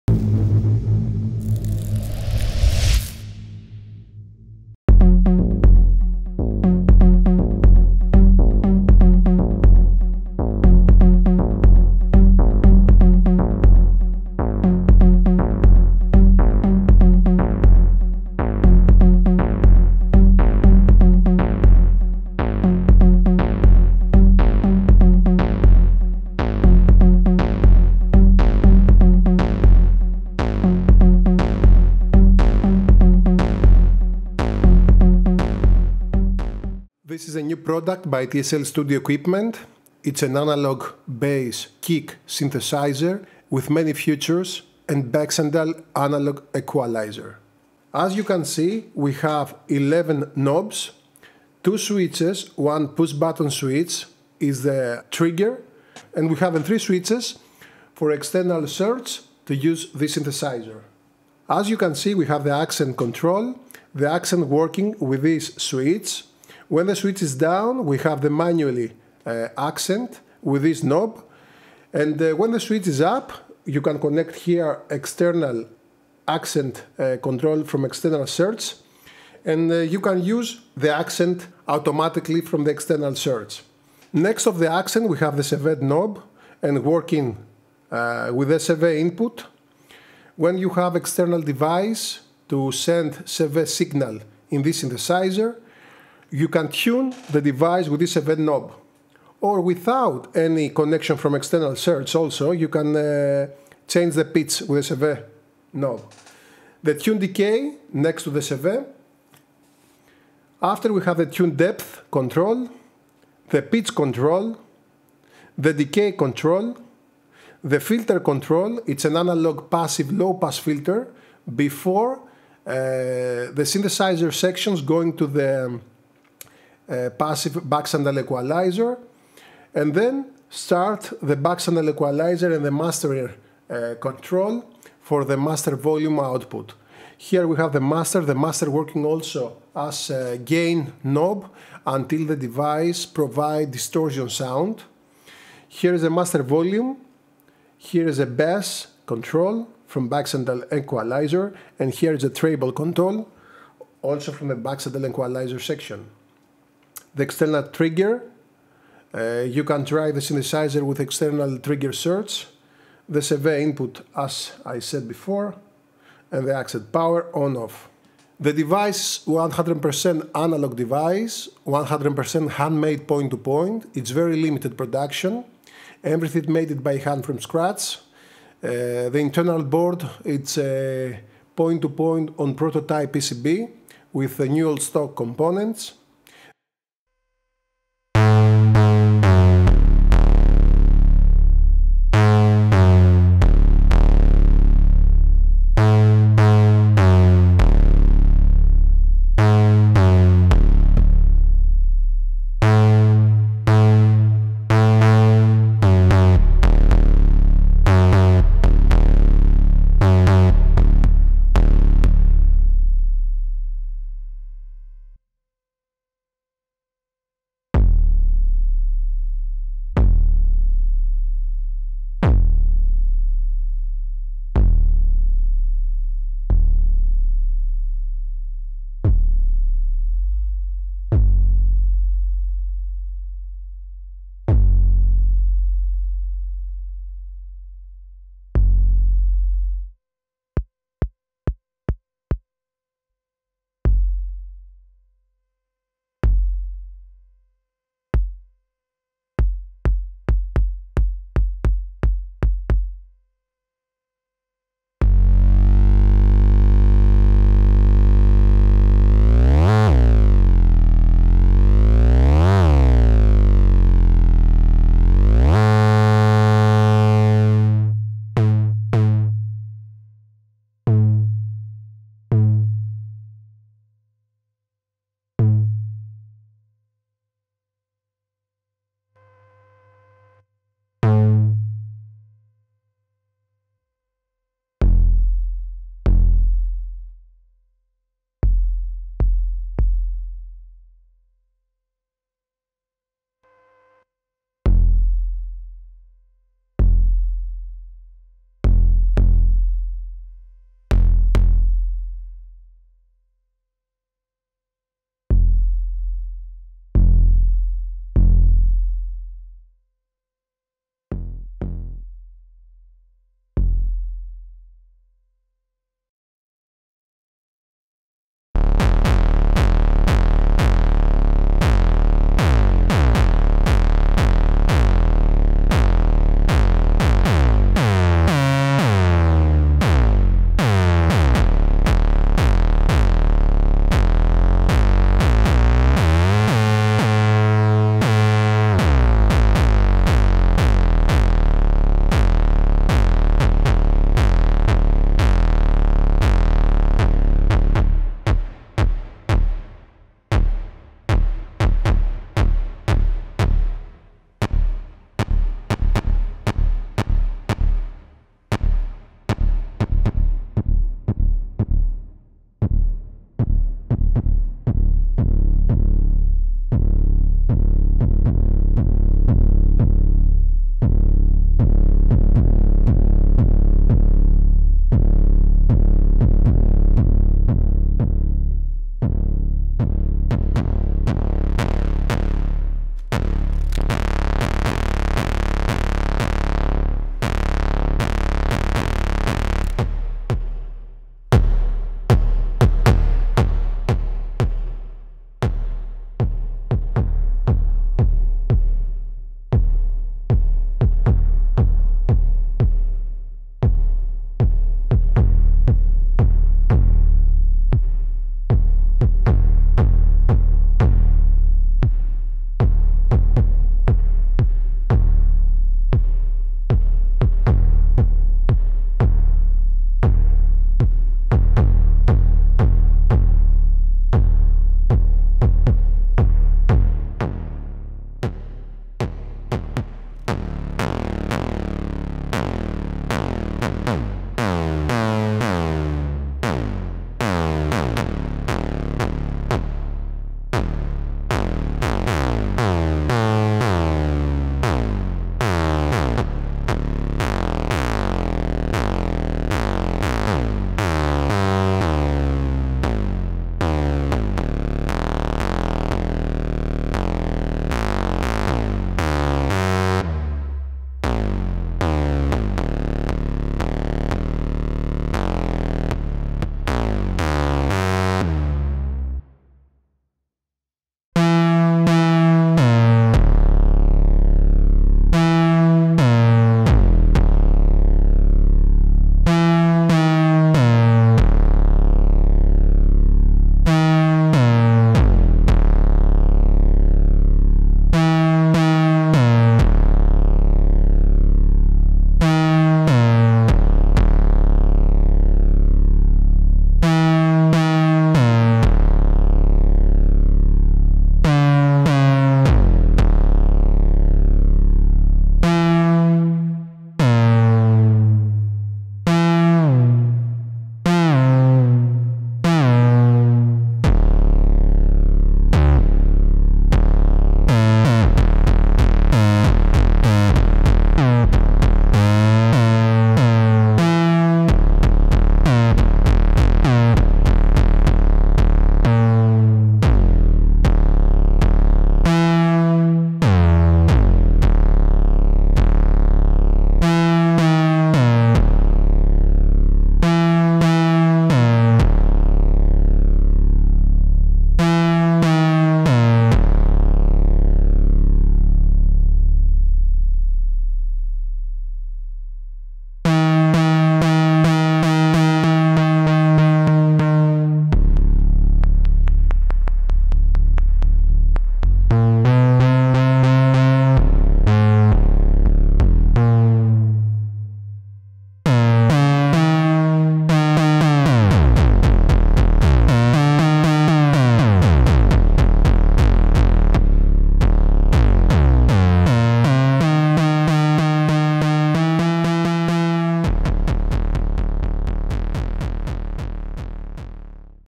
Unique Handmade Analog Bass & Kick Synthesizer with Baxandall EQ Synth by TSL Studio Equipment
♬ This is one of my New Unique Products:
Pure Analog Bass - Kick Synthesizer with Passive Baxandall EQ
KNOBS:
↣ ACCENT ( You can choose between the Internal ACCENT control or from the External Source ACCENT Socket front of the panel.
↣ CV Control to tune the sound as standalone or if you use it with external CV control from External Source.
↣ Tune Decay Control
↣ Tune Depth Control
↣  Pitch Control
↣ Decay Control
↣ Passive Low Pass Filter Before the Baxandall EQ stage
↣ Master Knob to Boost until distort the bass signal
↣ Master Volume Knob for the final export audio signal
↣ Bass Control Tone (Baxandall EQ)
↣ Treble Control Tone (Baxandall EQ)
SOCKETS:
↣ ACCENT IN Socket for External ACCENT Control
↣ CV IN Socket for the PITCH Control from External Source
↣ TRIGGER IN Socket to Trigger the Synth from External Source.
ALso the device it has Self Trigger Button front of the Panel.
100% Handmade Point to Point with NOS Compopnents on Prototype PCB and built in Handmade High Quality Pure Aluminum and Metal Enclosure.